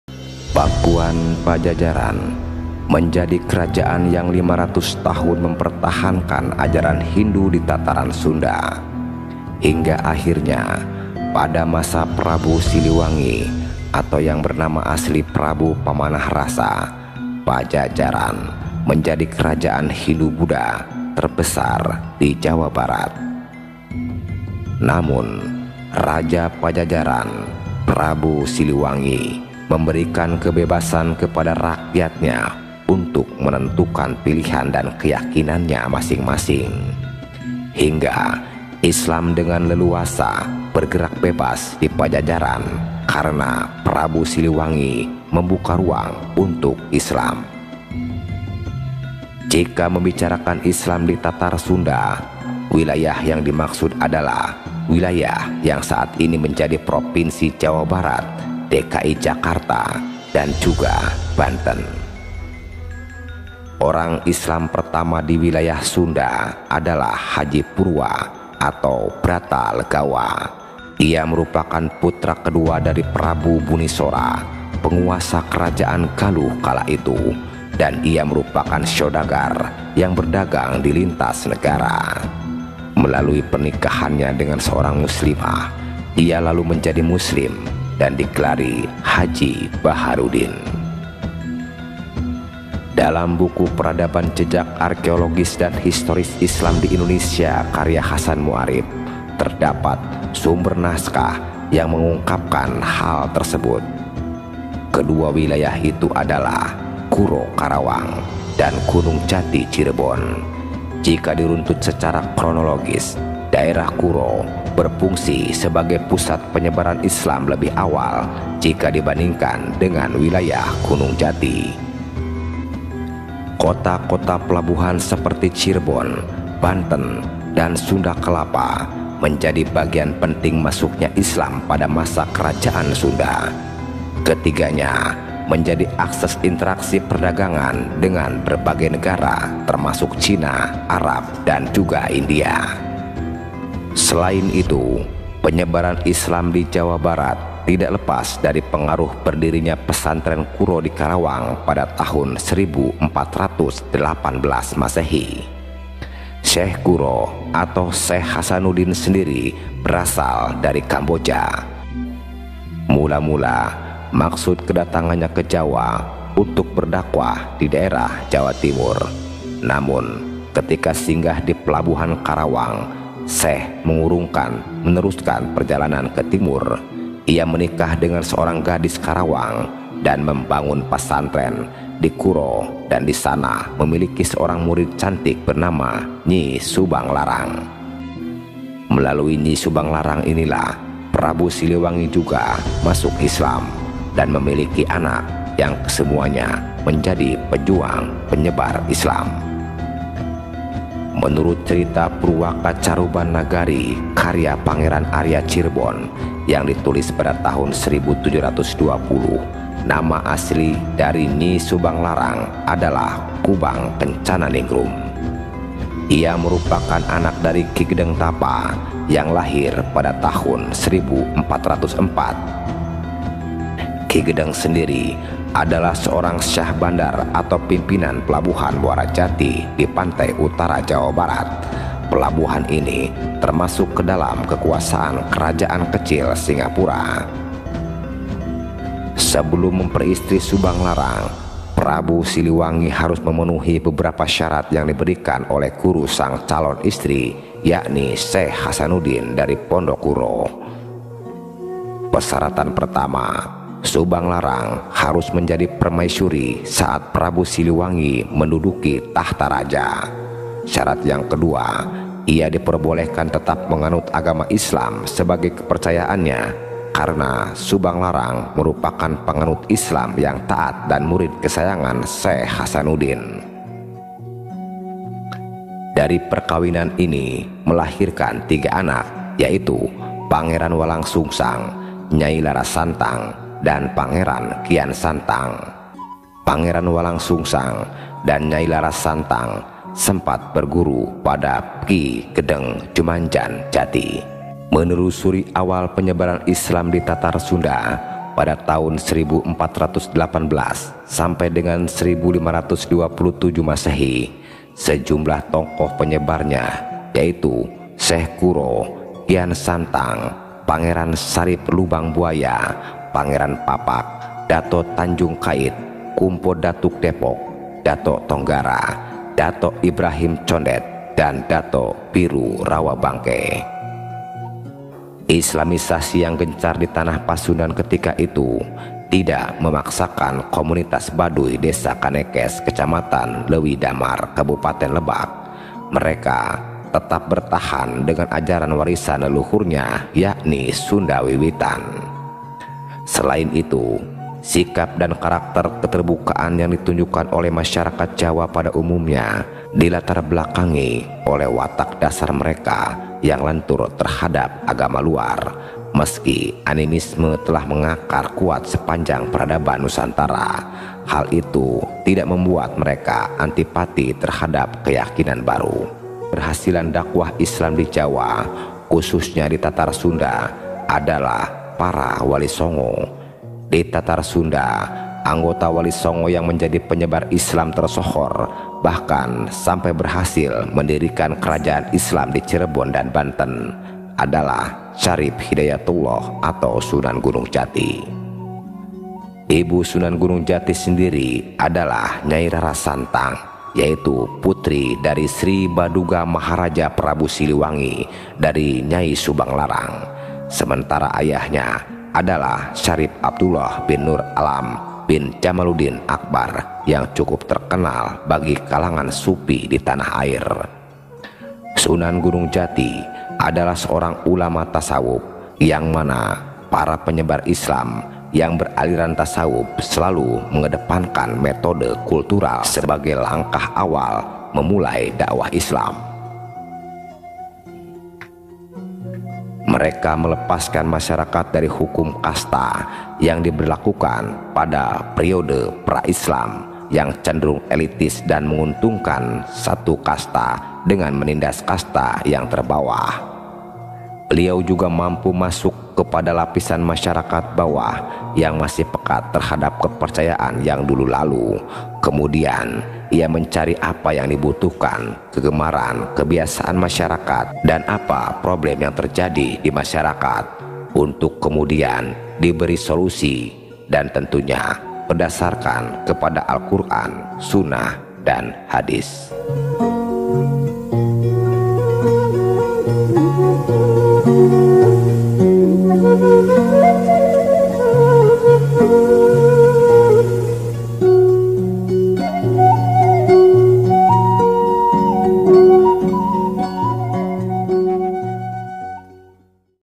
prabu siliwangi dan jejak islam di tanah sunda
prabu siliwangi adalah raja pluralis yg memberikan kebebasan kepada rakyatnya untuk memeluk agama apapun sesuai keyakinan mereka,dan ahirnya agama islam berkembang pesat di tatar sunda,hingga islam menjadi agama mayoritas sampai sekarang ini.